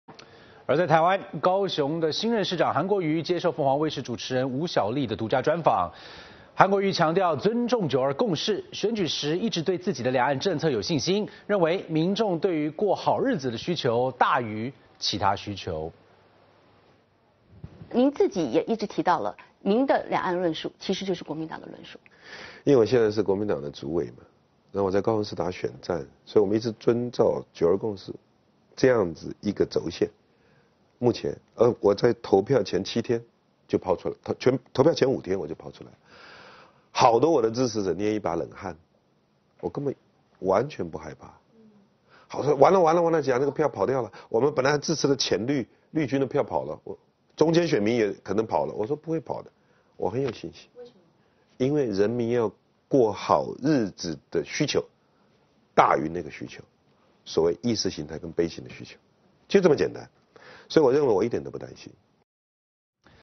韓國瑜接受鳳凰獨家專訪：尊重“九二共識” 得民眾支持 20190103
高雄新任市長韓國瑜接受鳳凰衛視主持人吳小莉的獨家專訪。韓國瑜強調，尊重“九二共識”，選舉時一直對自己的兩岸政策有信心，認為民眾“過好日子”的需求大於其他需求。

【PC端】点击链接，扫描首页二维码下载凤凰卫视官方App凤凰秀